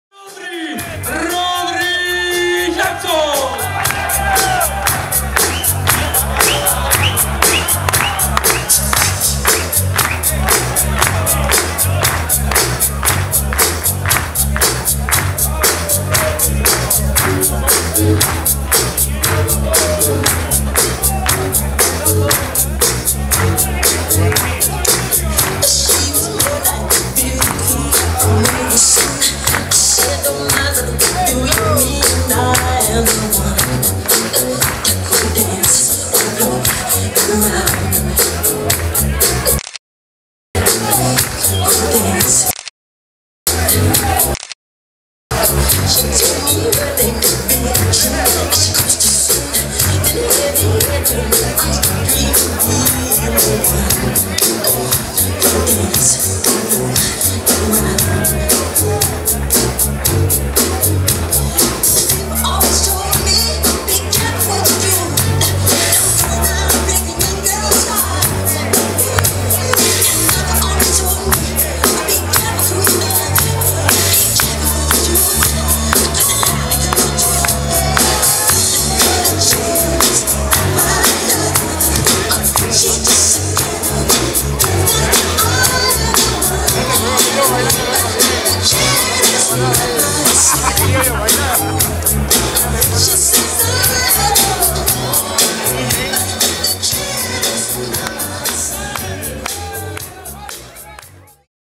Rodrigo Villagra - El Ramonazo 2019
Rodrigo baila un tema de Michael  Jackson en El Ramonazo 2019. Con Pepe Ramón y Tucán Poliche.